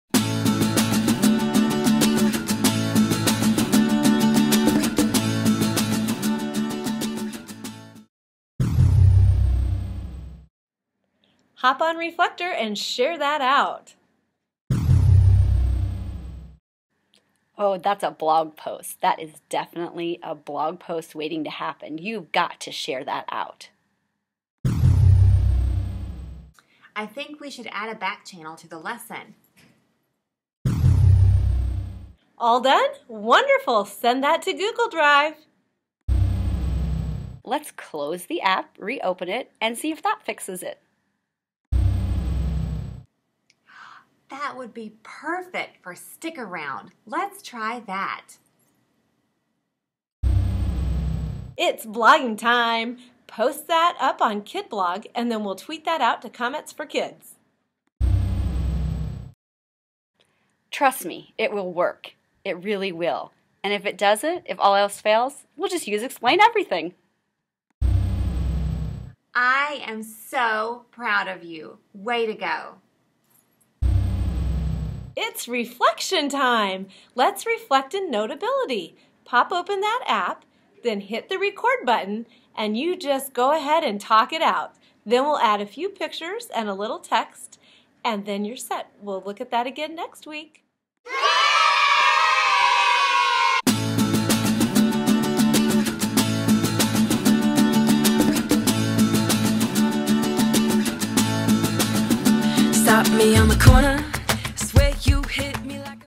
Things iPad Coaches Say
The t4tt team saying a few things that iPad coaches say!